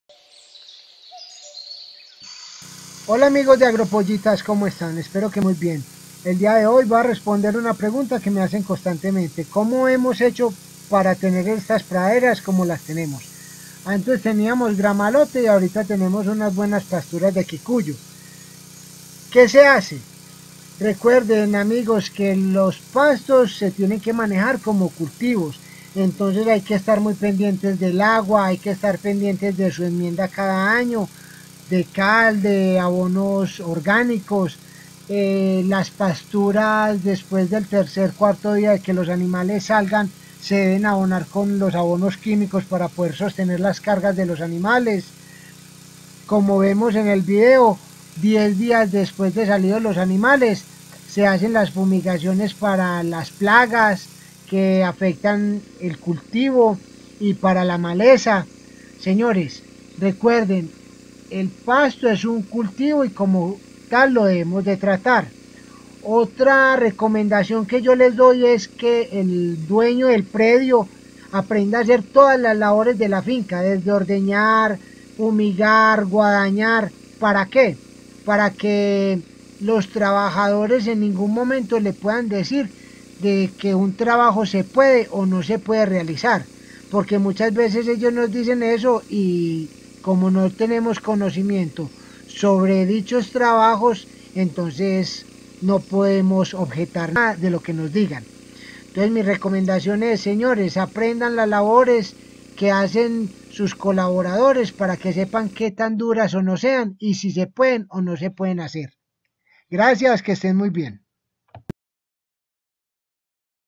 COMO MEJORAR LOS PASTOS
El pasto se debe manejar como un cultivo, es importante para el dueño del predio aprender las labores que se realizan en el campo.